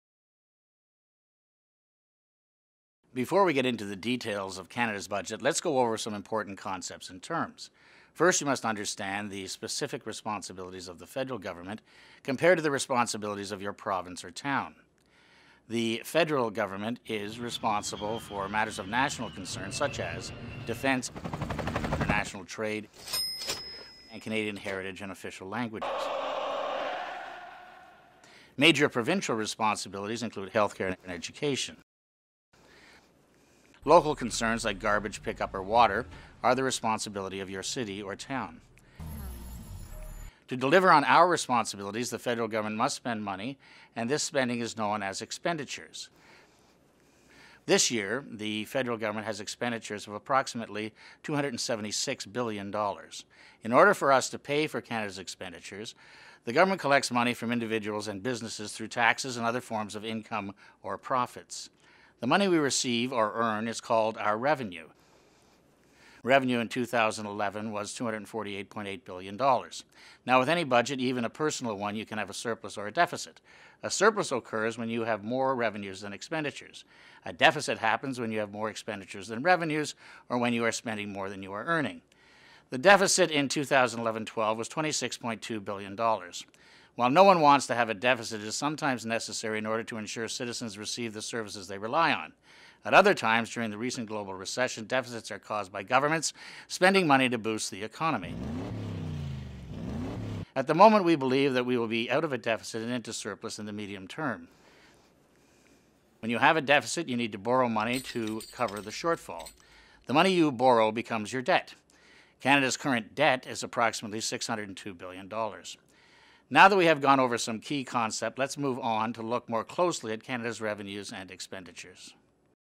[SBC 2013] Student Budget Consultation 2013 - Overview
For the first time, high schools across Canada are invited to participate in the Student Budget Consultation and advise the Minister of Finance on the nation's fiscal priorities.